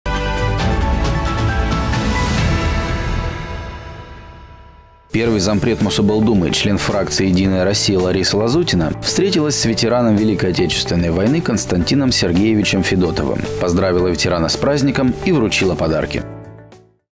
Встреча с ветераном
Первый зампред Мособлдумы, член фракции «Единая Россия» Лариса Лазутина встретилась с ветераном Великой Отечественной войны Константином Сергеевичем Федотовым.  Поздравила ветерана с праздником и вручила подарки.